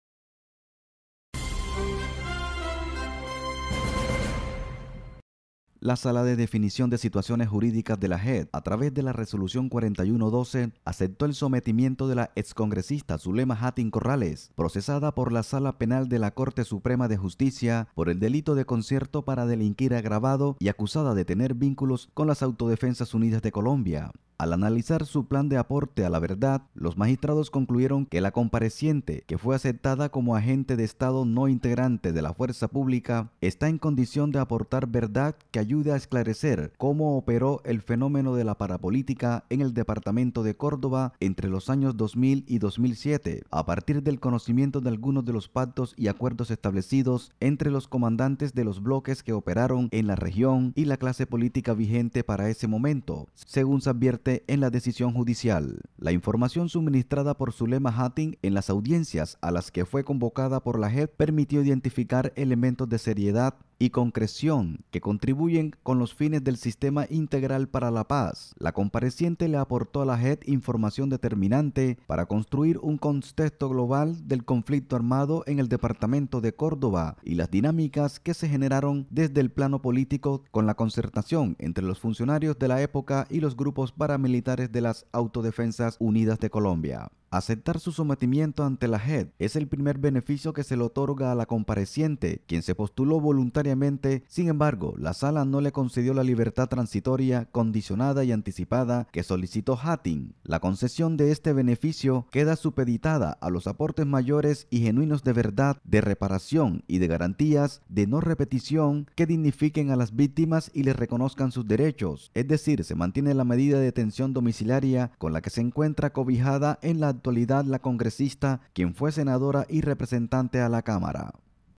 JEP aceptó sometimiento de Zulema Jattin, pero la mantiene en prisión domiciliaria
La Sala de Definición de Situaciones Jurídicas de la JEP, a través de la Resolución No.4112, aceptó el sometimiento de la excongresista Zulema Jattin Corrales, procesada por la Sala Penal de la Corte Suprema de Justicia por el delito de concierto para delinquir agravado y acusada de tener vínculos las Autodefensas Unidas de Colombia (AUC).